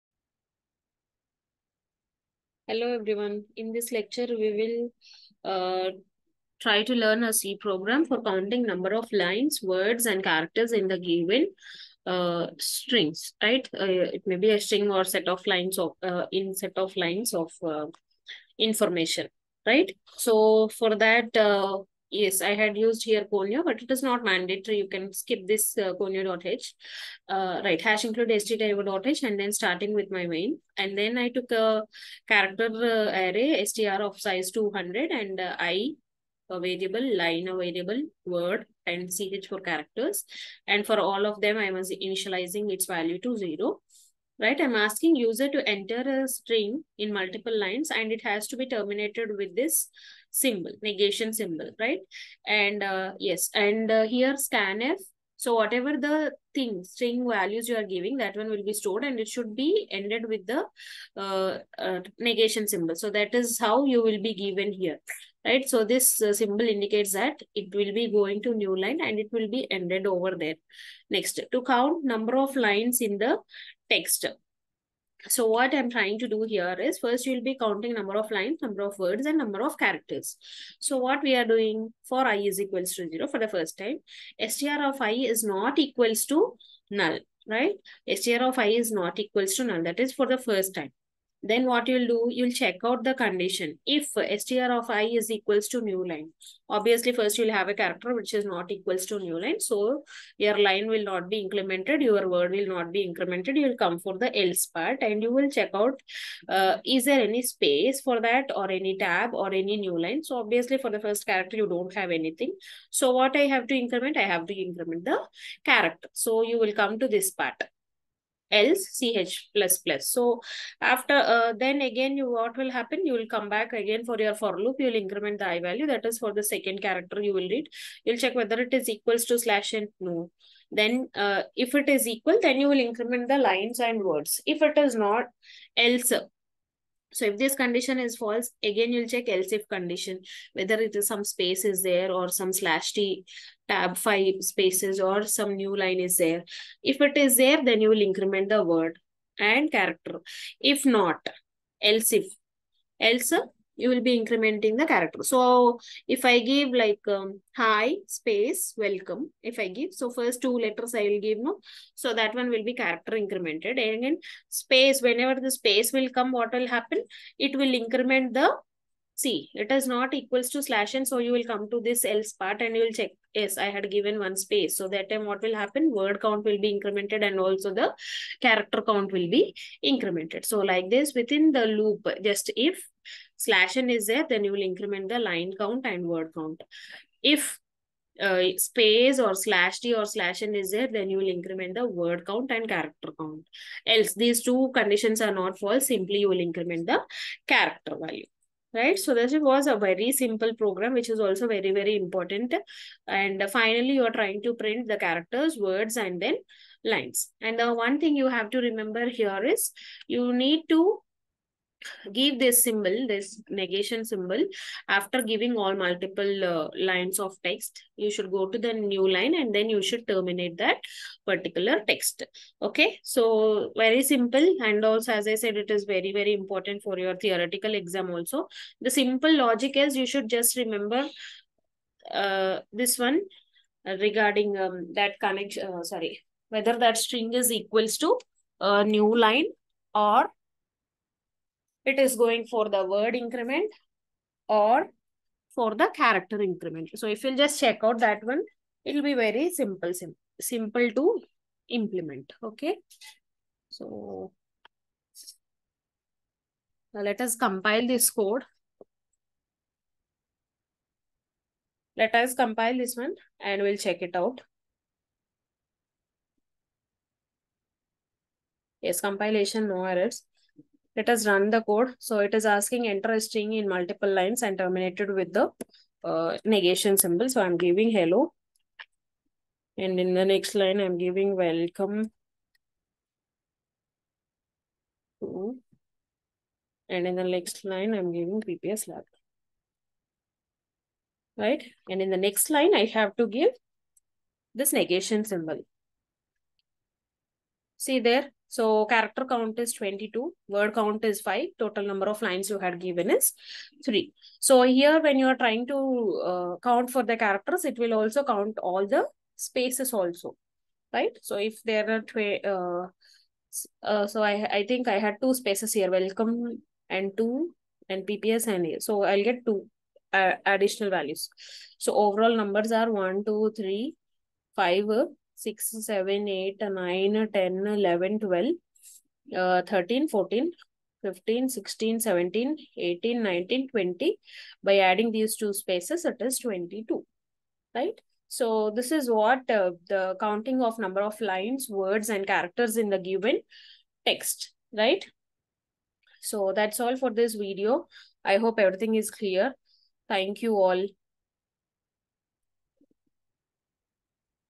35.C program to count no of characters, no of words, no of lines in given text |C Language Lab, PPS
C program to count no of characters, no of words, no of lines in given text |C Language Lab, PPS